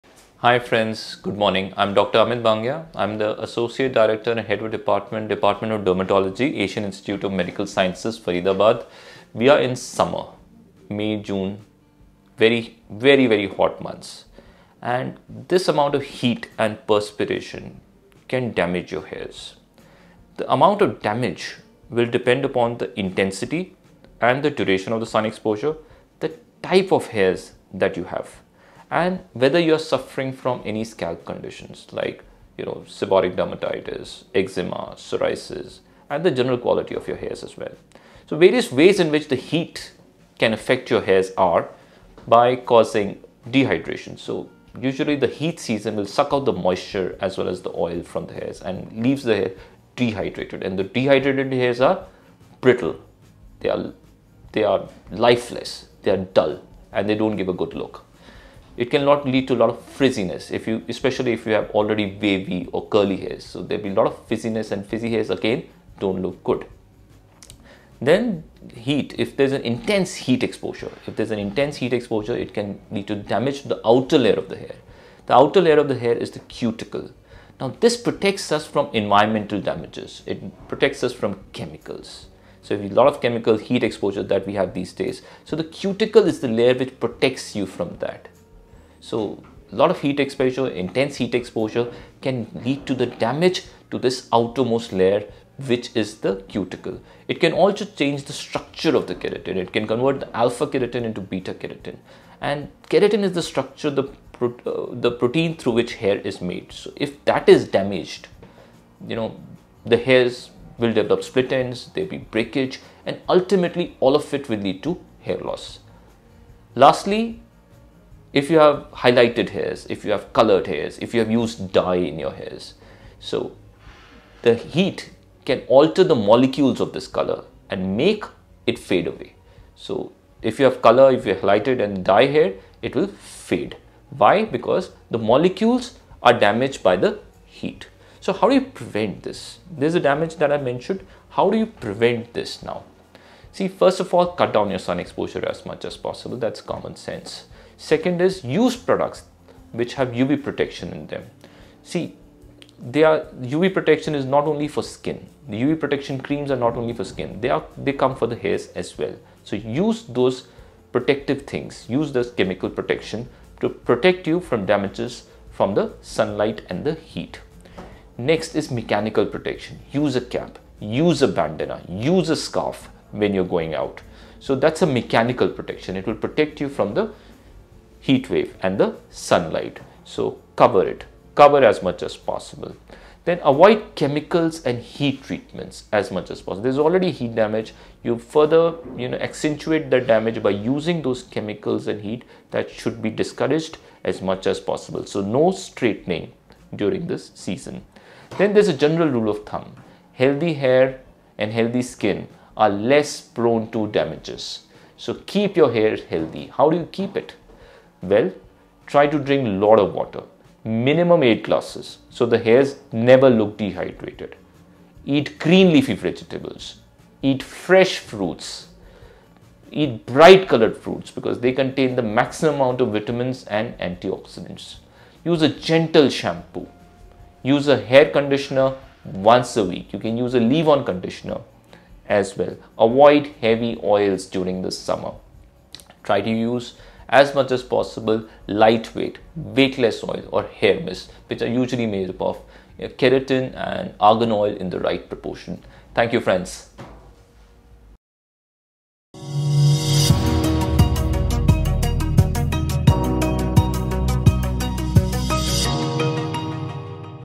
Summer heat can harm your hair | How to prevent hair from sun damage | TimesXP
Summer causes a lot of damage to the hair because of exposure to intense UV rays. Dr. Amit Bangia, talks about the right ways to protect your hair from heat in the summer season!